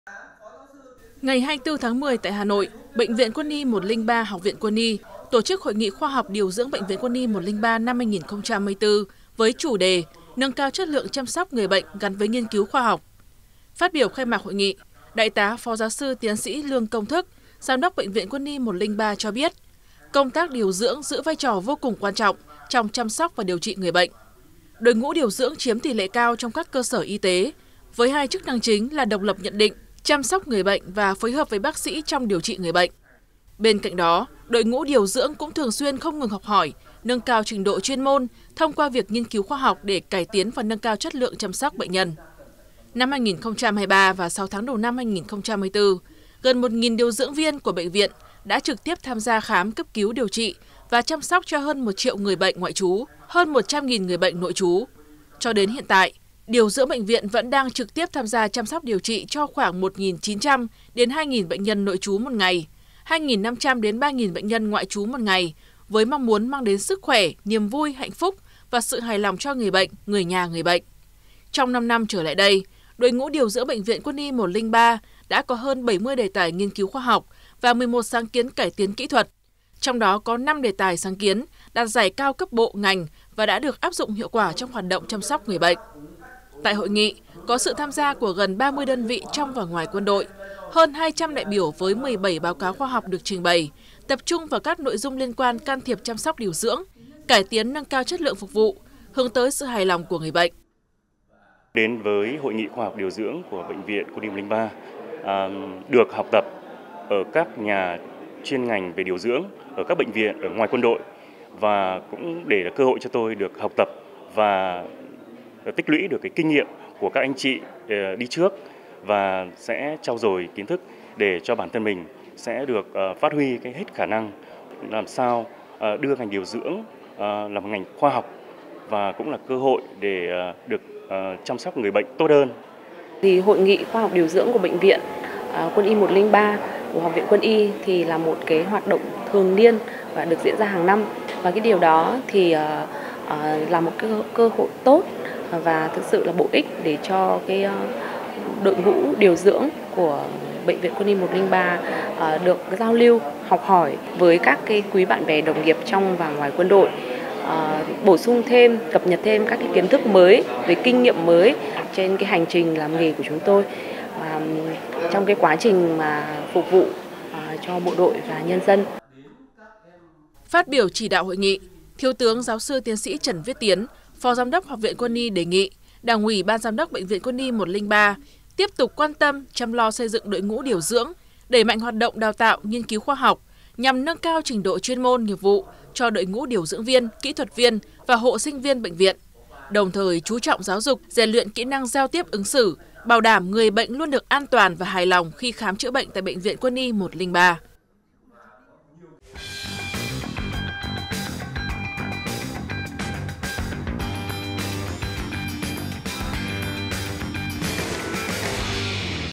Bệnh viện Quân y 103 nâng cao chất lượng chăm sóc người bệnh
Báo QĐND - Ngày 24-10, tại Hà Nội, Bệnh viện Quân y 103 (Học viện Quân y) tổ chức Hội nghị Khoa học Điều dưỡng Bệnh viện Quân y 103 năm 2024 với chủ để “Nâng cao chất lượng chăm sóc người bệnh gắn với nghiên cứu khoa học”.